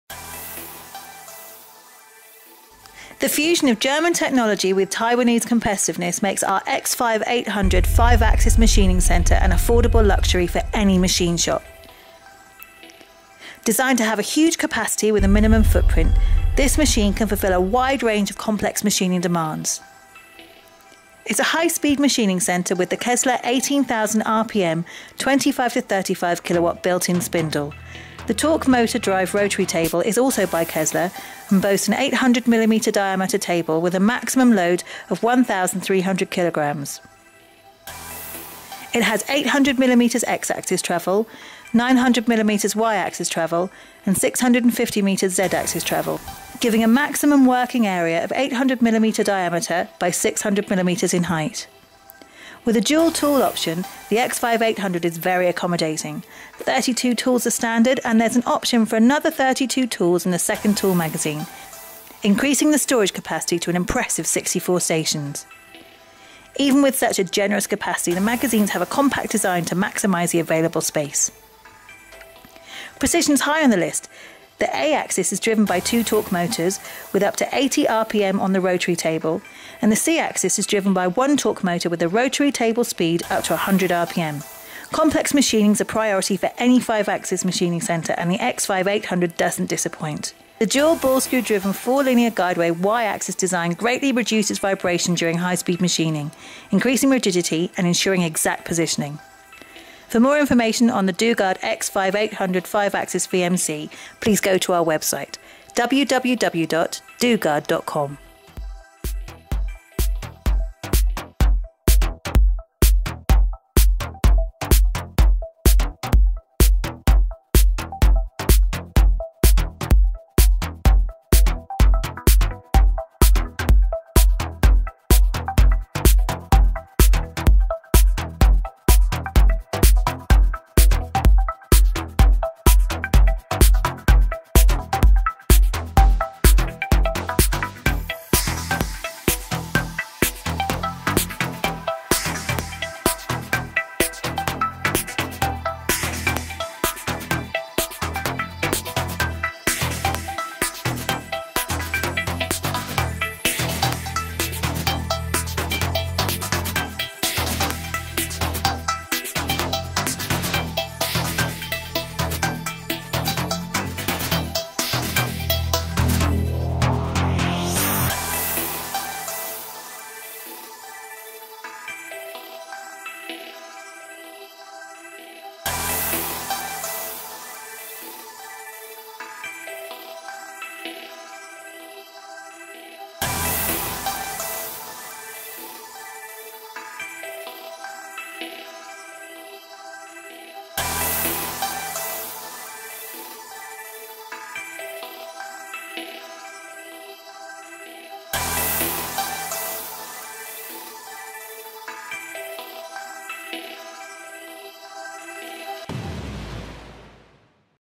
Dugard X5 800 Five Axis VMC
Dugard X5-800 Five Axis Vertical Machining Centre
Machine Specification
Made in Taiwan
Heidenhain TNC640 control
Kessler Torque Table
Working table size Ø800mm
Workpiece dimensions Ø800 x H600mm
Table load 1300kg
Spindle nose to table surface 100~750mm
T slot (width x no) 14mm x 7
A axis rotation range ±120°
A axis rotation speed 80rpm
C axis 360°
C axis rotation speed 100rpm
X axis travel 800mm
Y axis travel 900mm
Z axis travel 650mm
Rapid speed 48/48/48 m/min
Cutting feed rate 15/15/15 m/min
VDI3441 positioning 0.008mm
VDI3441 repeatability 0.004mm
Spindle taper HSK 63
Spindle power (S1/S6 40%) 25/35kW
Spindle speed 18,000rpm
Max spindle torque (S1/S2 40%) 86/130Nm
Tool storage capacity 32 ATC (64,128)
Max tool diameter with adjacent tool 75mm
Max tool diameter without adjacent tool 125mm
Max tool length 350mm
Max tool weight 7kg
Air capacity 5.5 kg/cm2
Power capacity 80 kVA
Cutting fluid capacity 750 L
Floor space (L x W x H) 5600 x 2600 x 3600mm
Machine weight 20,000kg

Standard Equipment
Swarf conveyor and bin
Through spindle coolant 20 bar
Heavy duty high quality cast iron frame
Heavy duty roller guideways
Manual handwheel
Ethernet connection
USB programme storage
PC flashcard programme storage
Rigid tapping
Helical interpolation
Counterbalance head
Fully enclosed guarding
High power coolant system
Chip flushing system
32 x set of pull studs
Automatic lubrication
Machine Options
64 tools
128 tools
24,000rpm spindle, 20/25kW (built in)
Renishaw spindle probe OMP60
Renishaw tool setting probe TS 27 R
Renishaw non-contact tool setter NC4-S